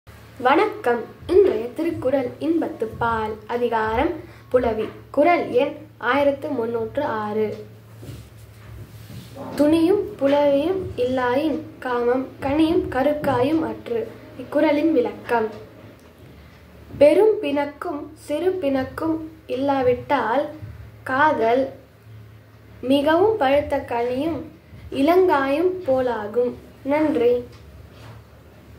குறள் எண் - 1306 திருக்குறள் இன்பத்துப்பால் அதிகாரம் - 131. புலவி
துனியும் புலவியும் இல்லாயின் காமம்
கனியும் கருக்காயும் அற்று